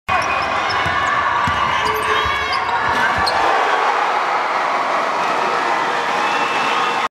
Phelps anota ante Osos de Guadalajara
Phelps contibuyo con 41 unidades siendo el jugador el partido contra Osos de Guadalajara.